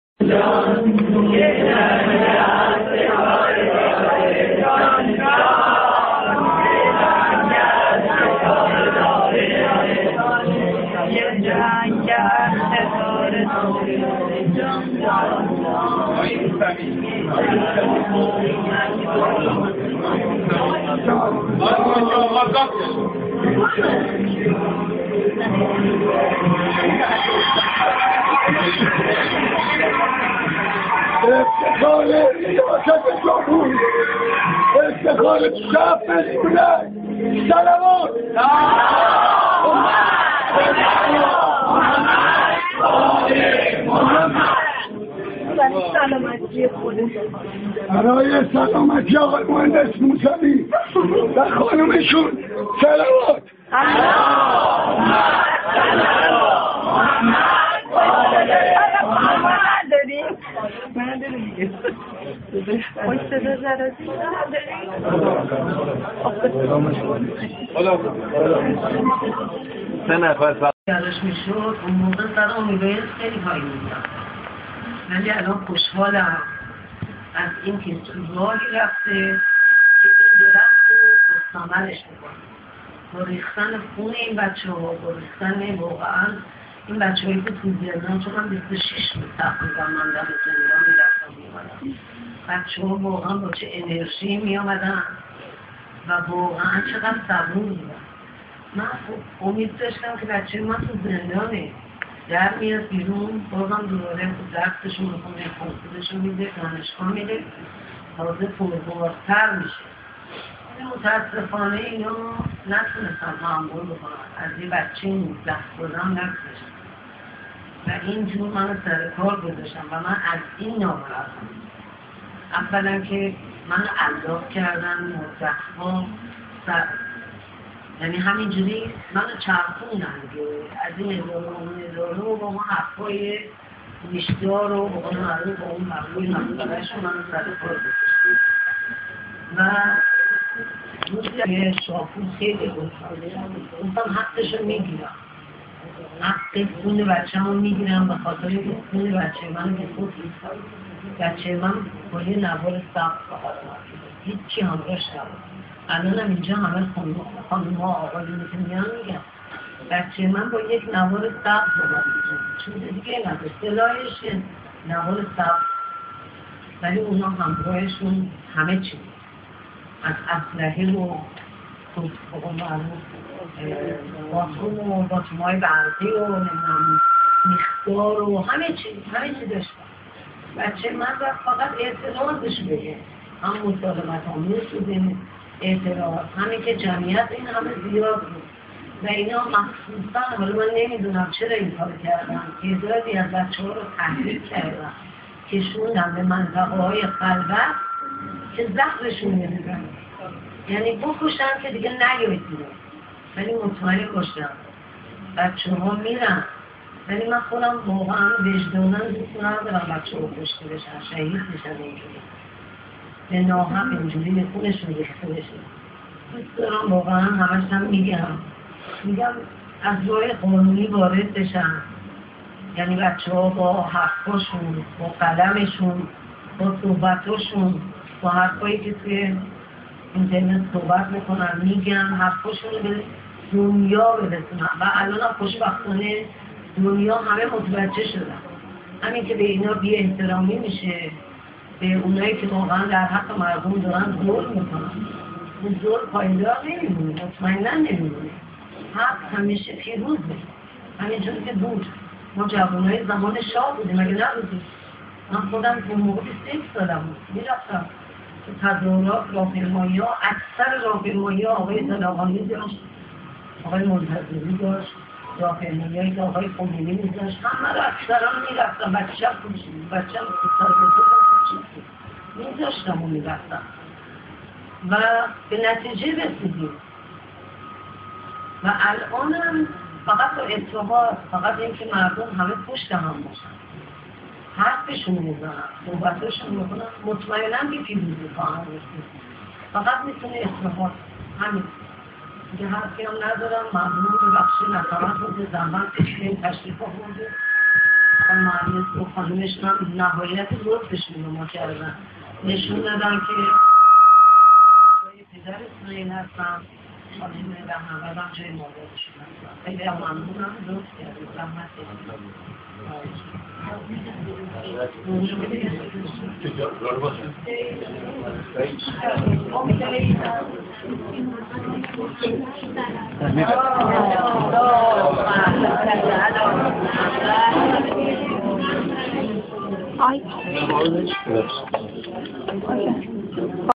Mousavi in Sohrab Arabi house,july 14 ,9pm
Mir Hossein Mousavi today (2009,July 14) visited the family of Sohrab Arabi 19 yes old who killed in the protest by police .His family had no news about his whereabouts for 26 days.